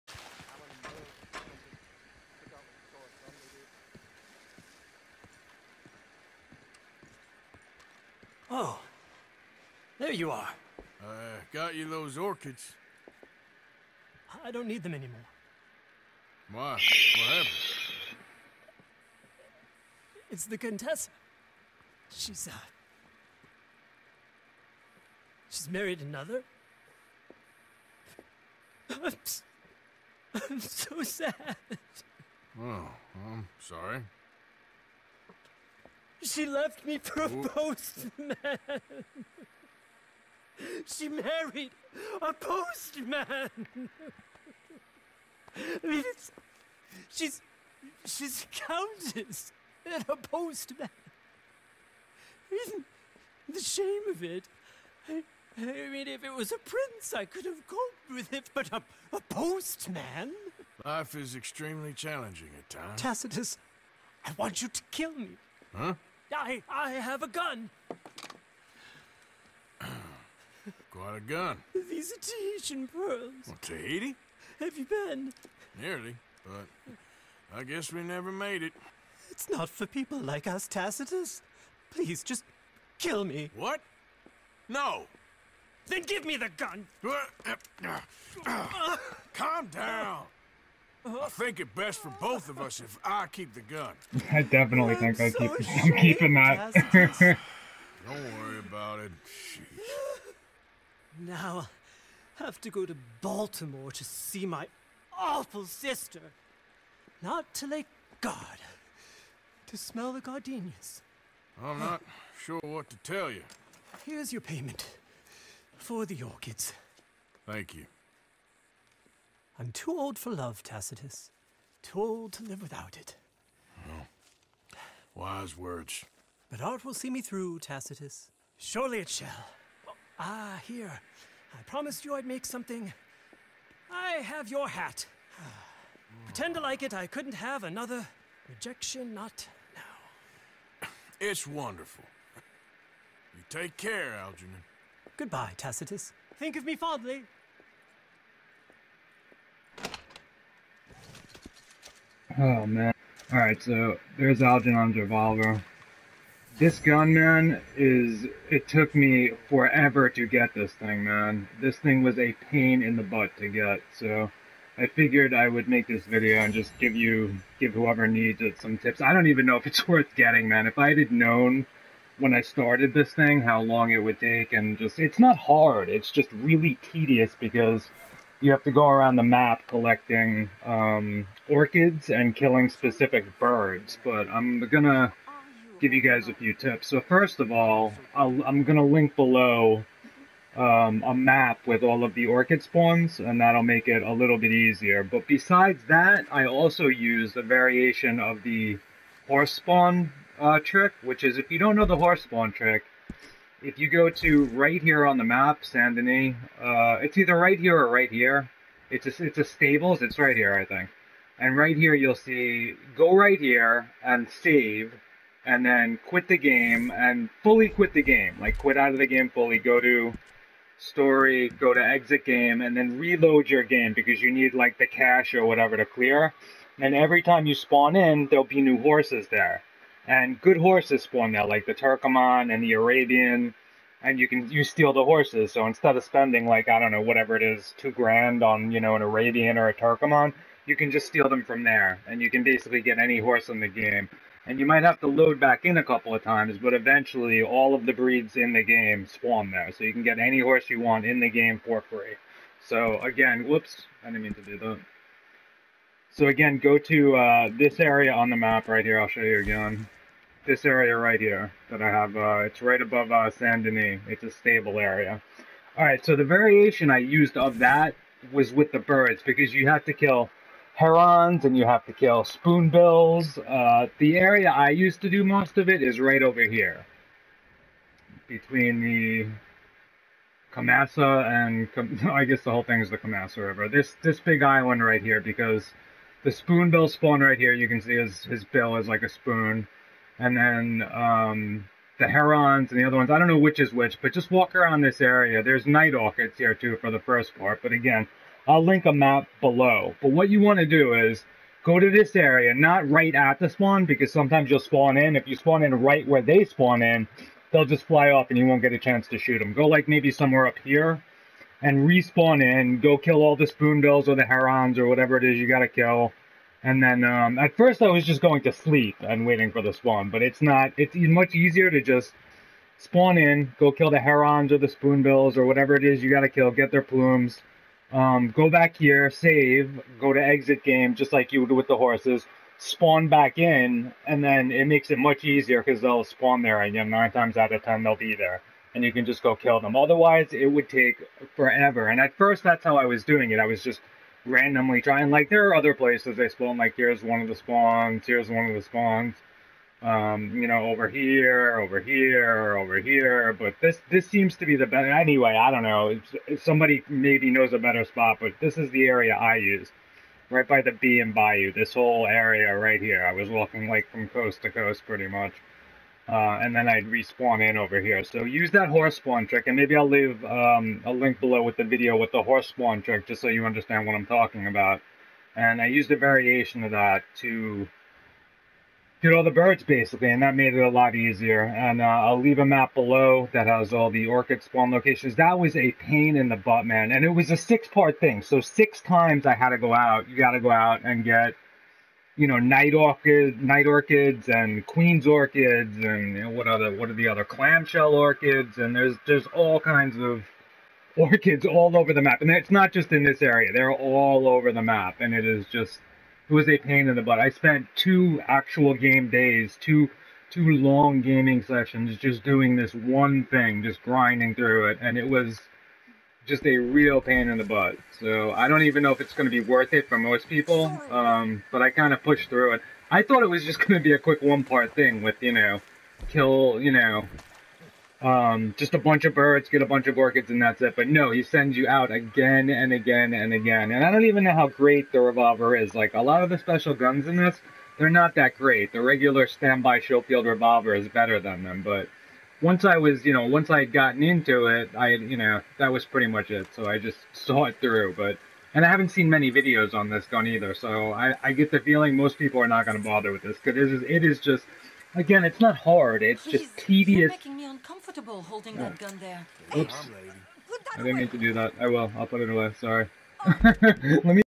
Algernon’s Revolver Tips
The orchid spawns-

Oh, do the horse spawn in the afternoon, and when it’s not raining for best results. Ride it to a secluded area with your main horse following you, and saddle it where nobody can see you. Usually you don’t get caught stealing it, you get caught saddling it, so saddle it discreetly. Happy hunting.